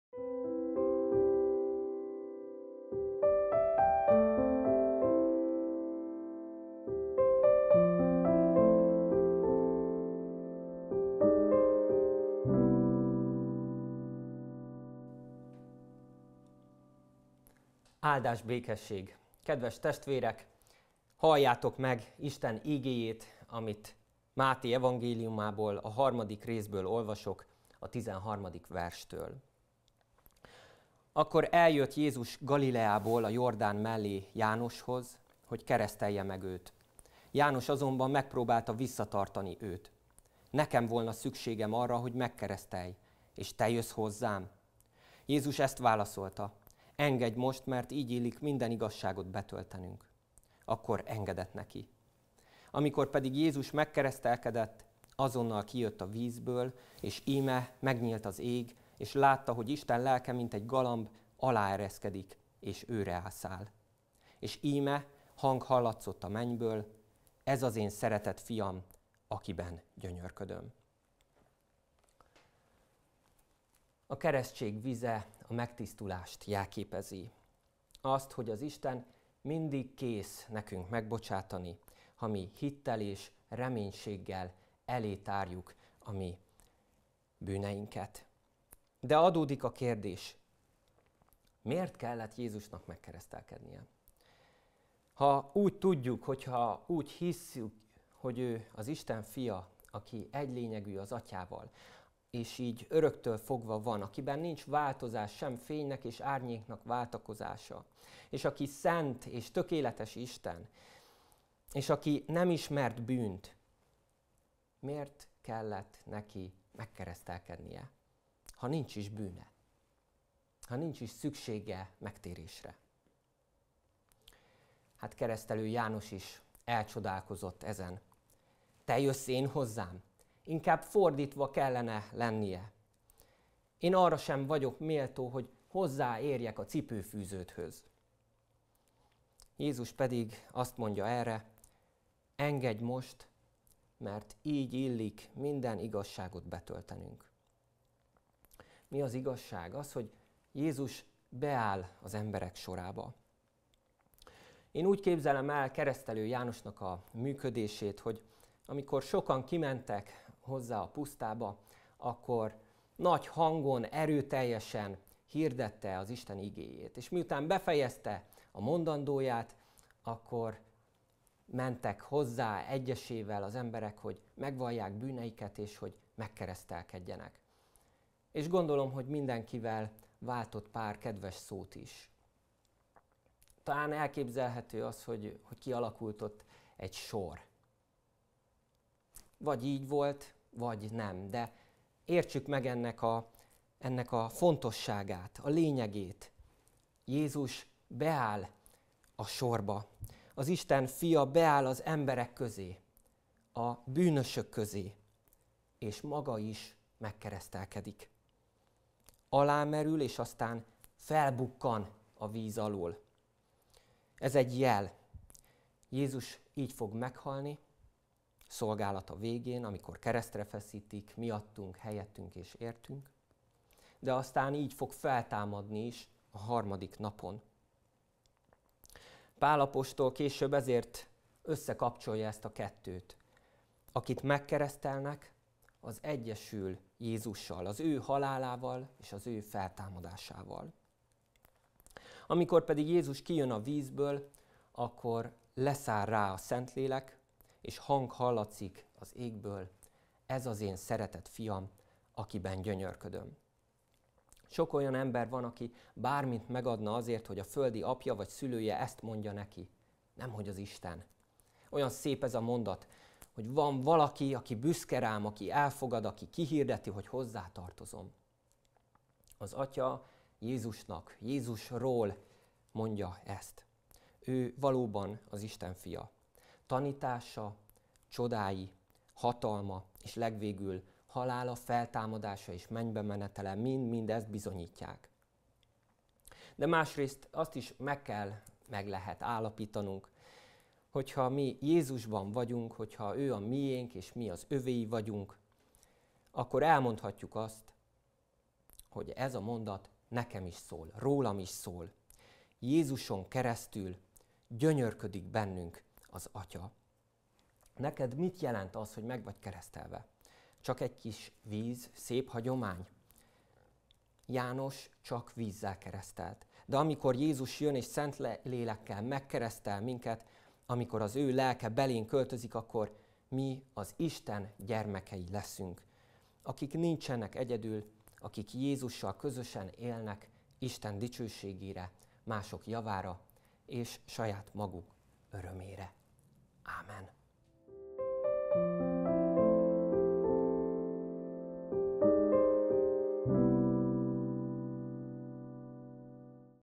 Igeidő - 2024.06.25.
Akkor eljött Jézus Galileából a Jordán mellé Jánoshoz, hogy keresztelje meg őt. János azonban megpróbálta visszatartani őt: Nekem volna szükségem arra, hogy megkeresztelj, és te jössz hozzám? Jézus ezt válaszolta: Engedj most, mert így illik minden igazságot betöltenünk. Akkor engedett neki. Amikor pedig Jézus megkeresztelkedett, azonnal kijött a vízből, és íme, megnyílt az ég, és látta, hogy Isten Lelke mint egy galamb aláereszkedik, és őreá száll. És íme, hang hallatszott a mennyből: Ez az én szeretett Fiam, akiben gyönyörködöm.
(Máté evangéliuma 3. fejezet 13-17. vers)
Napindító áhítat Szabó Istvánnal